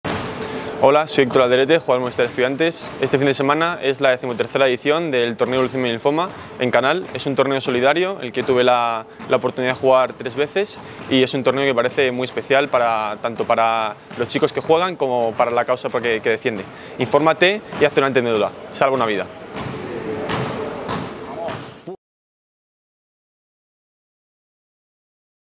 Héctor Alderete te invita al XIII
El jugador del Movistar Estudiantes, quién formó parte del Quinteto Ideal del Torneo FLL en dos ediciones consecutivas (2015 y 2016), y que cinco años más tarde, ya formaría parte del primer equipo del Movistar Estudiantes, con apenas 19 años, te invita al XIII Torneo de Baloncesto de la Fundación Leucemia y Linfoma, que se celebrará este fin de semana en el Pabellón Polideportivo Canal de Isabel II de Madrid.

Muchas gracias, Héctor, por formar parte del EquipoMédula🧡